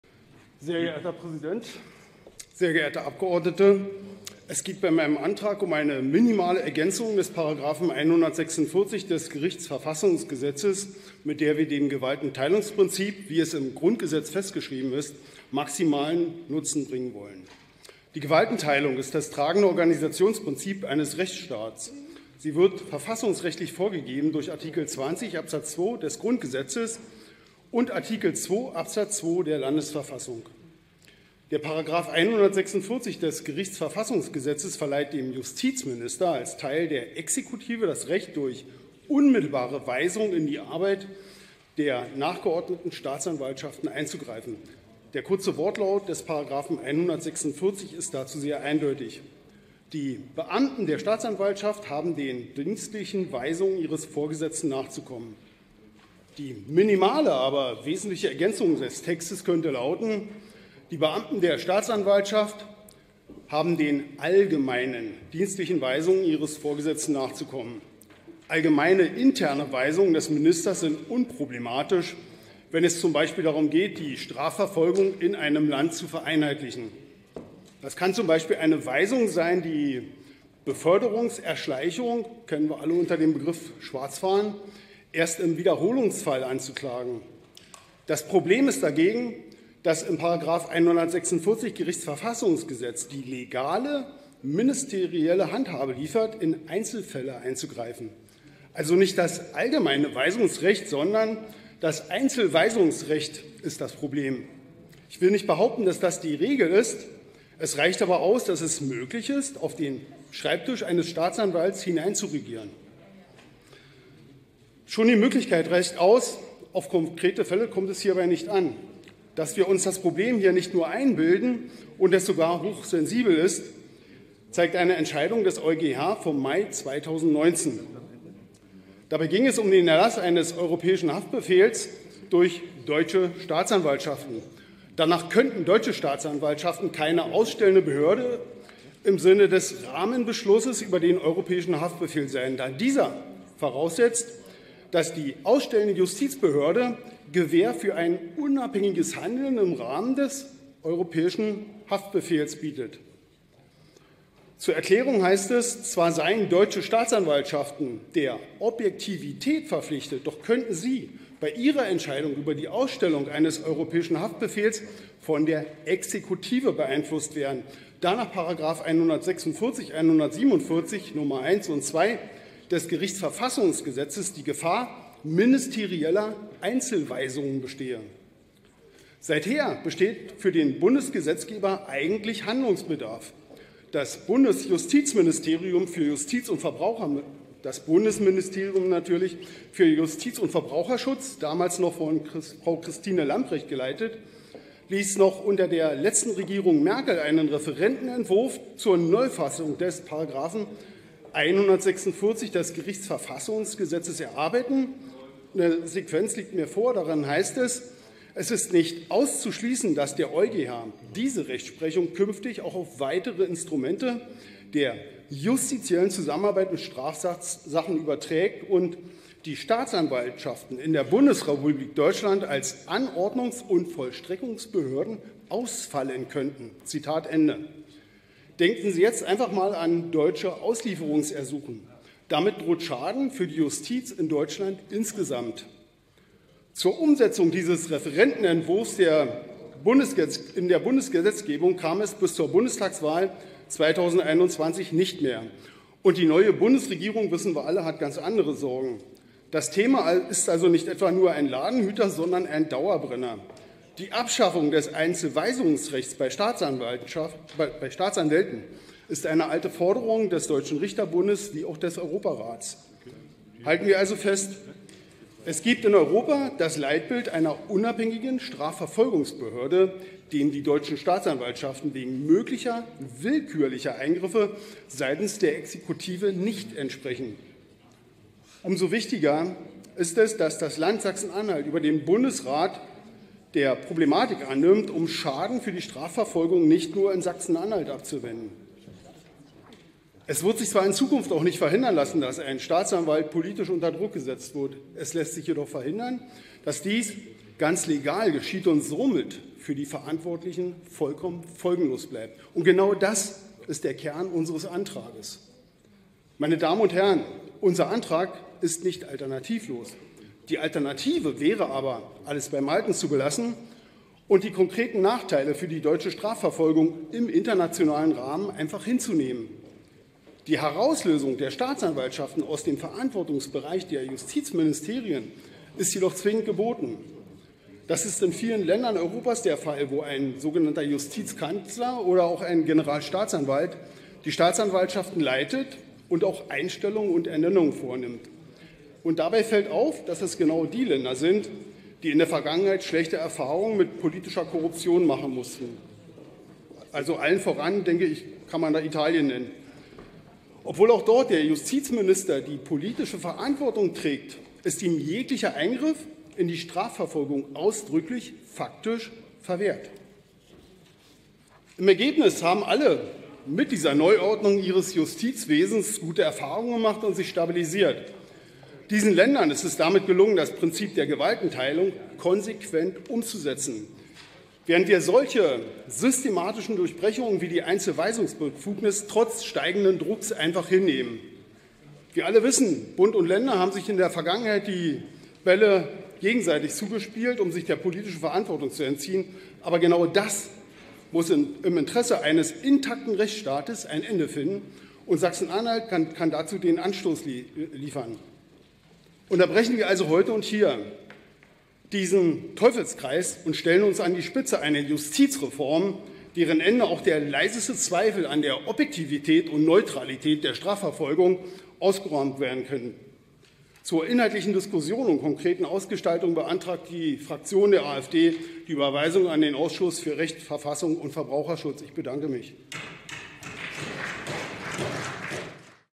Prinzip der Gewaltenteilung konsequent umsetzen
Gewaltenteilung ist das tragende Organisationssystem eines Rechtsstaates, mahnt MdL FrankOttoLizureck. Das Weisungsrecht darf nicht in Einzelfälle eingreifen. Staatsanwaltschaften dürfen in Rechtsentscheidungen nicht vom Justizminister angewiesen werden.